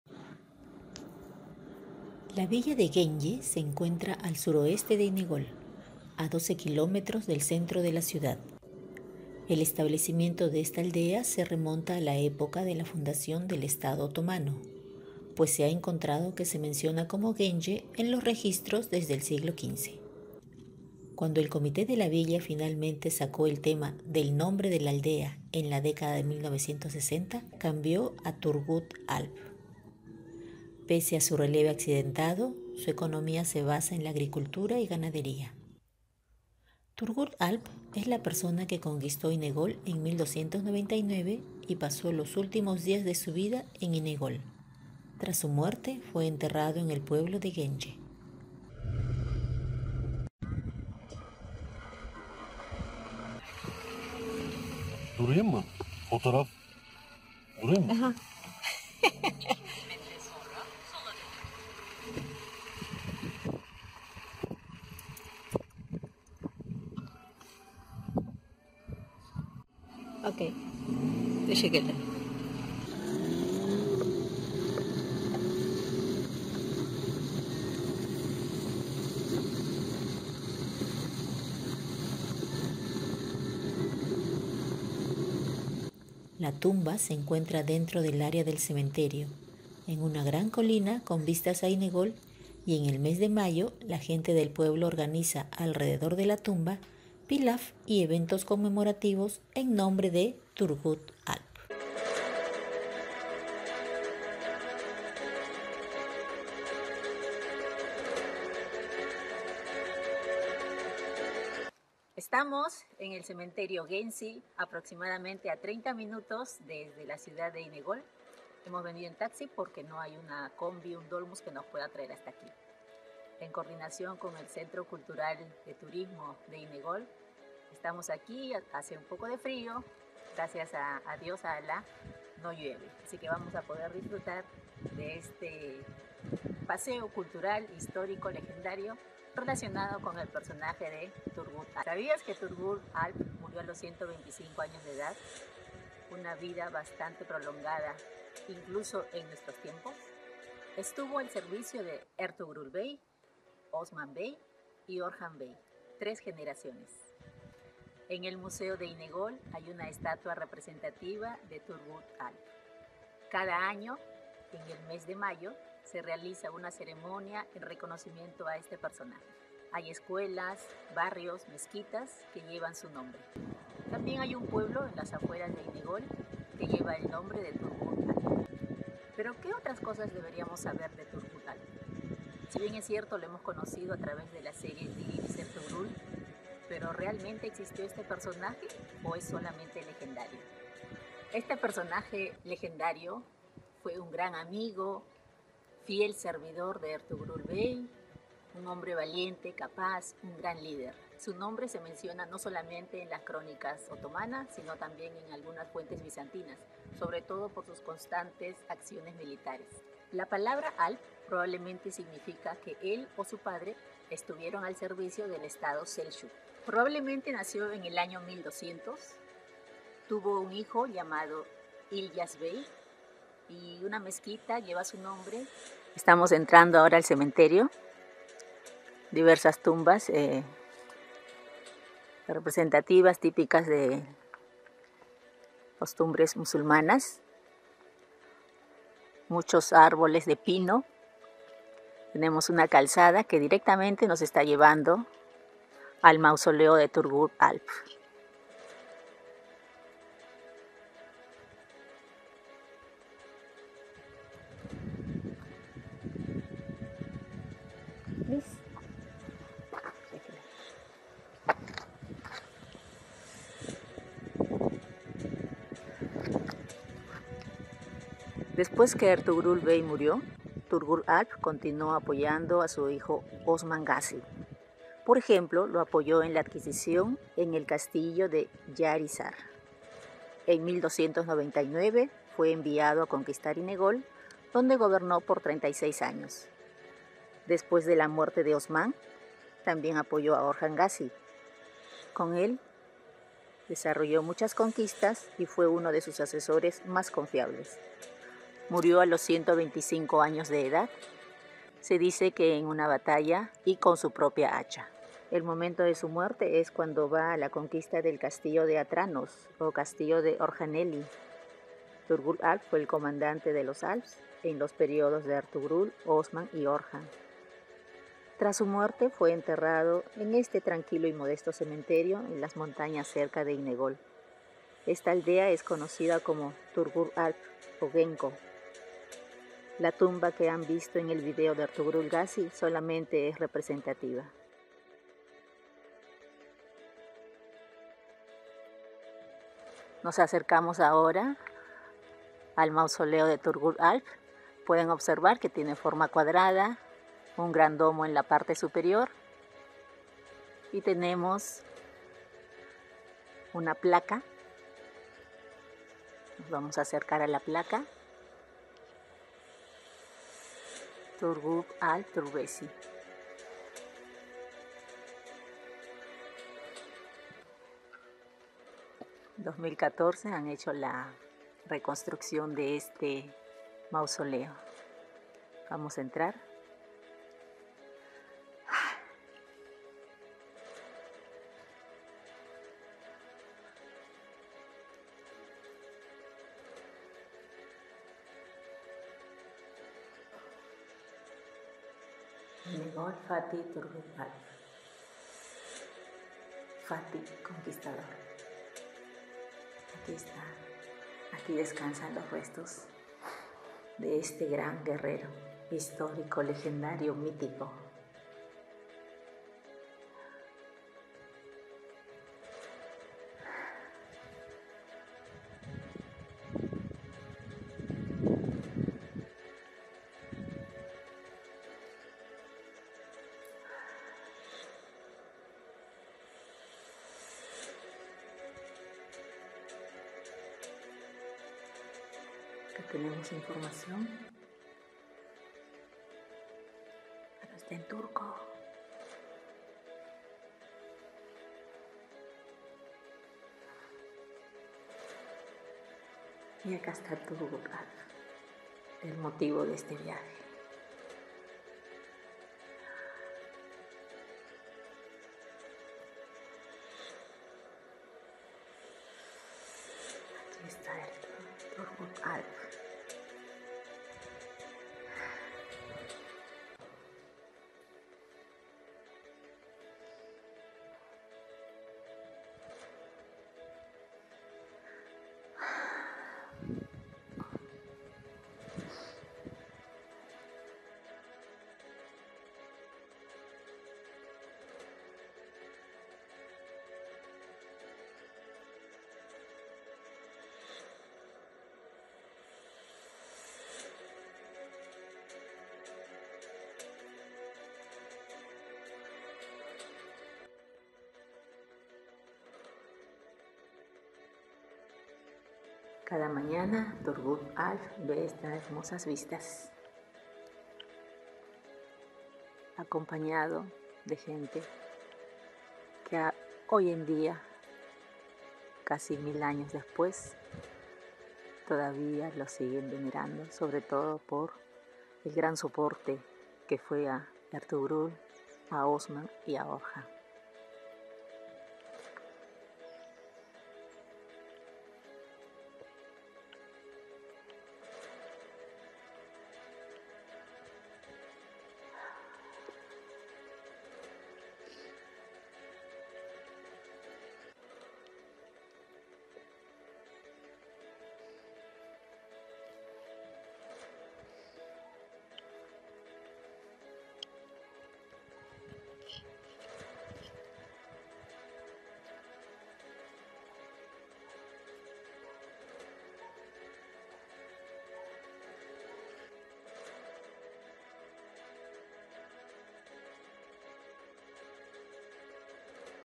Turgut Alp , mausoleo en la villa de Gence, Inegol-Bursa.
Turgut Alp fue un gran colaborador de Ertugrul Bey, Osman y Orhan Gazi. reconocido históricamente por sus hazañas en batalla , por su fuerza, valentía, lealtad y habilidad en el manejo del hacha . Desempeñó  importantes funciones  que a la fecha es considerado un personaje de gran influencia en la historia de la formación de Turquía. ¡Ven, vive la experiencia!